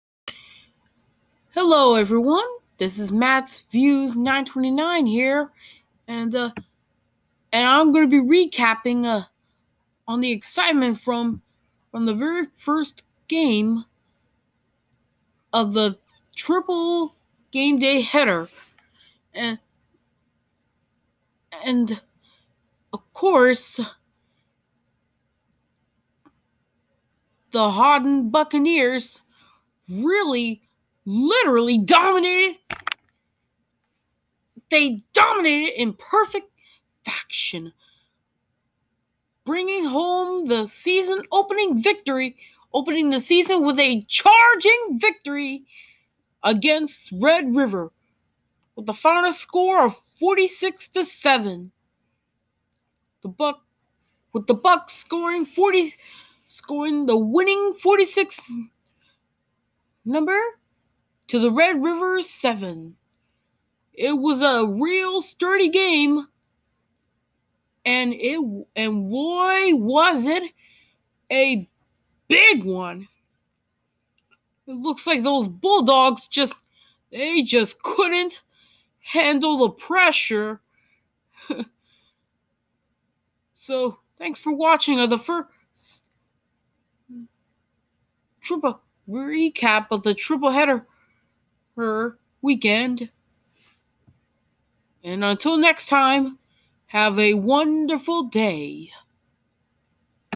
Matthew Talks About : Haughton Buccaneers vs. Red River Bulldogs Recap!
This is me recapping the game of the Buccaneers against the Bulldogs!